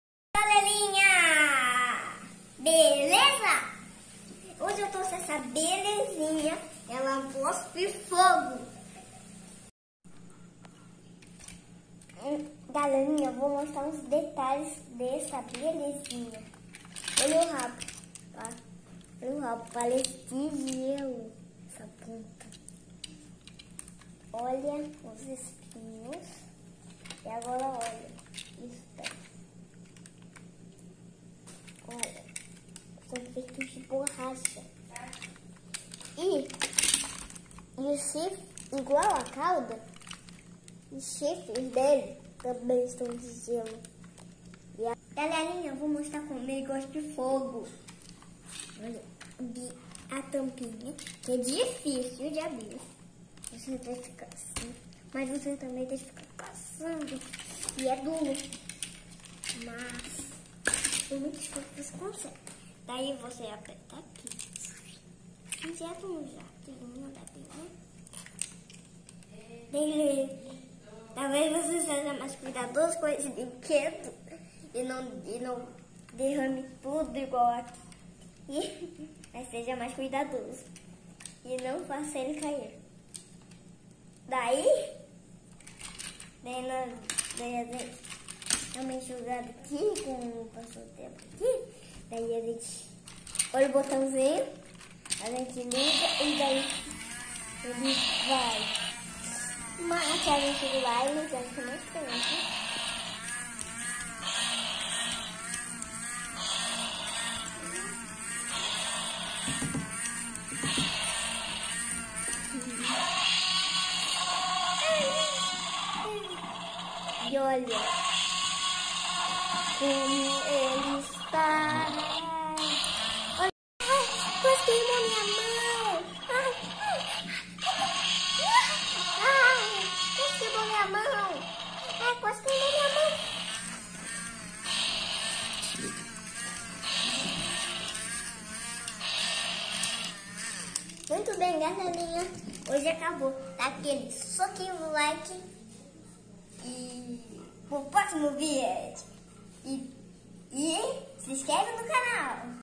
DRAGÃO VOADOR de Brinquedo COSPE FOGO e ANDA SOZINHO
neste vídeo mostro, meu incrível dragão voador, que cospe fogo e anda sozinho,brinquedo muito bonito e divertido.
 e não se esqueça do soquinho no like que me ajuda muito .

toy dragon (dragon breathes fire)
dragão dinossauro verde